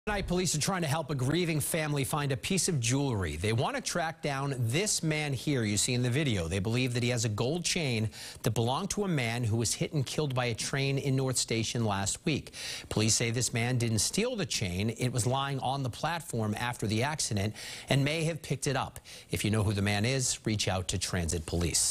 Police look for man who may have jewelry that belonged to man killed by train
Transit Police are looking to help a grieving family find a piece of jewelry by tracking down the man who may have it.